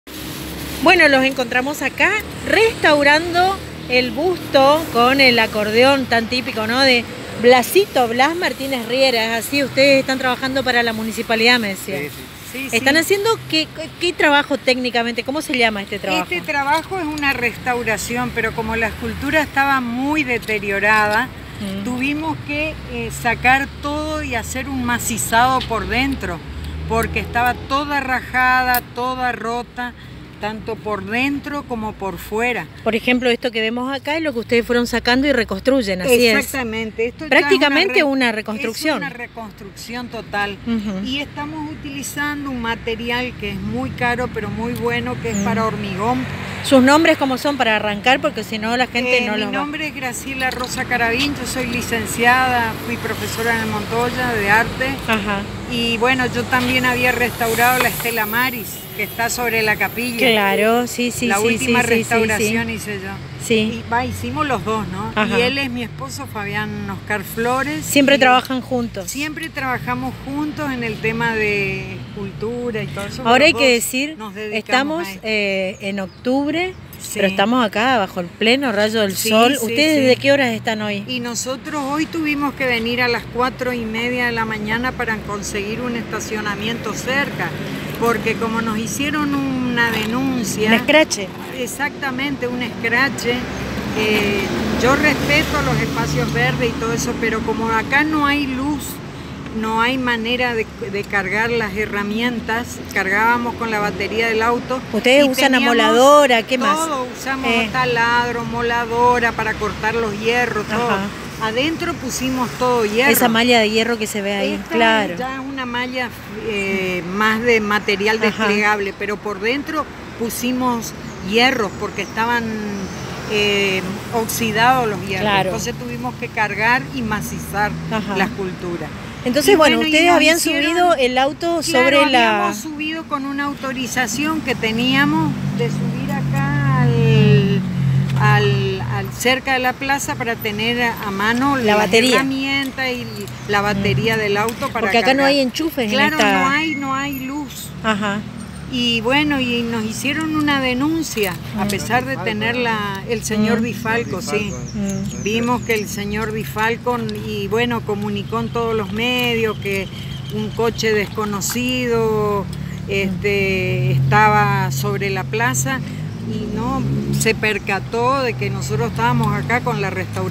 Artistas restauran el busto de Blas Martínez Riera
Posadas